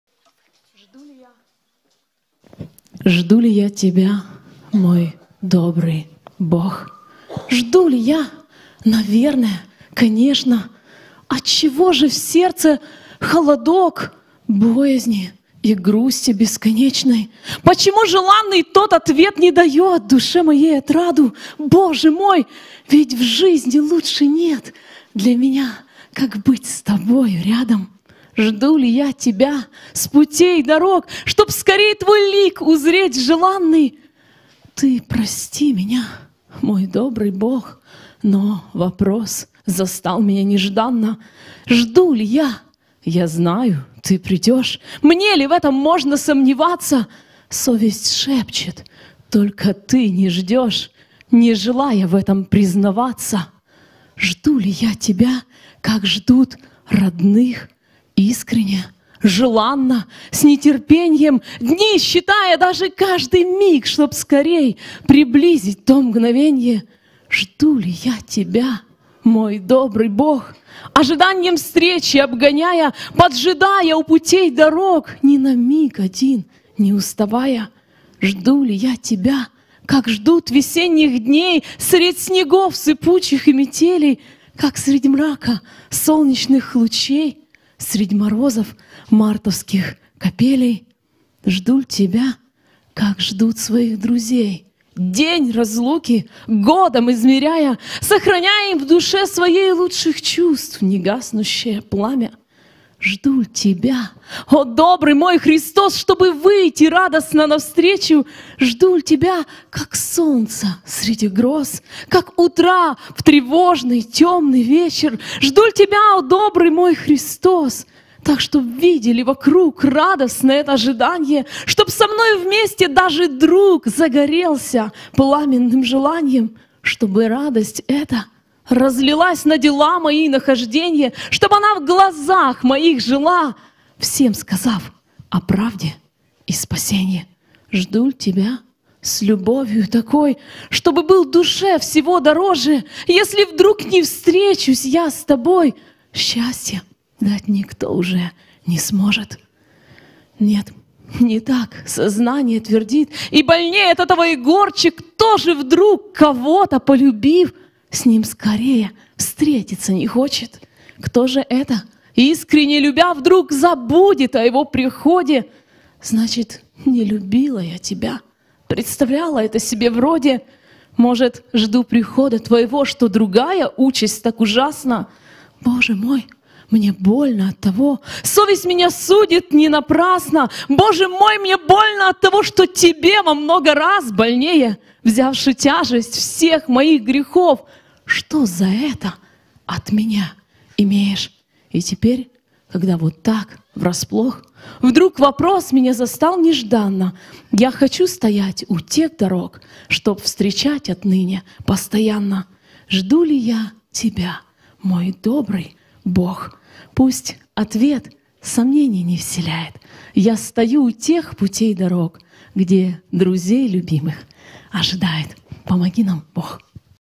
Жду ли я Тебя, мой добрый Бог? Стих-вопрос
Стихотворение об ожидании второго пришествия Христа, читает Светлана Кучеренко.

Фрагмент богослужения Киевской церкви «Жива надія» 19 декабря 2021 года.

 Жду ли я Тебя, мой добрый Бог?
Жду ли я? – Наверное, конечно.
Отчего же в сердце холодок
Боязни и грусти бесконечной?

Почему желанный тот ответ
Не дает душе отраду?
Боже мой, ведь в жизни лучше нет
Для меня, как быть с тобою рядом!

Жду ли я Тебя с путей – дорог,
Чтоб скорей Твой лик узреть желанный?
Ты прости меня, мой добрый Бог,
Но вопрос застал меня нежданно.

Жду ли я? Я знаю, Ты придешь.
Мне ли в этом можно сомневаться?
Совесть шепчет: только ты не ждешь,
Не желая в этом признаваться.

Жду ли я Тебя, как ждут родных:
Искренно, желанно, с нетерпеньем,
Дни считая, даже каждый миг,
Чтоб скорей приблизить то мгновенье?

Ожиданьем встречи обгоняя,
Поджидая у путей – дорог,
Ни на миг один не уставая?

Жду ли я Тебя, как ждут весенних дней
Средь снегов сыпучих и метелей,
Как средь мрака – солнечных лучей,
Средь морозов – мартовских капелей?

Жду ль Тебя, как ждут своих друзей?
День разлуки годом измеряя,
Сохраняя им в душе своей
Лучших чувств негаснущее пламя?

Жду ль Тебя, о добрый мой Христос,
Чтобы выйти радостно навстречу?
Жду ль Тебя, как солнце среди гроз,
Как утра в тревожный темный вечер!

Жду ли так, чтоб видели вокруг
Радостное это ожиданье;
Чтоб со мною вместе даже друг
Загорелся пламенным желаньем?

Чтобы радость эта разлилась
На дела мои и на хожденья,
Чтоб она в глазах моих жила,
Всем сказав о правде и спасенье.

Жду ль Тебя с любовию такой,
Чтобы был душе всего дороже?
Если вдруг не встречусь я с Тобой,
Счастья дать уже никто не сможет.

Нет, не так! – сознание твердит,
И больней от этого и горьче:
Кто же вдруг, кого-то полюбив,
С ним скорее встретиться не хочет?

Кто же это, искренно любя,
Вдруг забудет о Его приходе?
Значит, не любила я Тебя –
Представляла это себе вроде.

Может, жду прихода Твоего,
Что другая участь так ужасна?
Боже мой, мне больно оттого –
Совесть меня судит не напрасно.

Что Тебе во много раз больнее
Взявши тяжесть всех моих грехов,
Что за это от меня имеешь?

И теперь, когда вот так, врасплох,
Вдруг вопрос меня застал нежданно,
Я хочу стоять у тех дорог,
Чтоб встречать отныне постоянно.

Пусть ответ сомненья не вселяет:
Я стою у тех путей – дорог,
Где друзей любимых ожидают.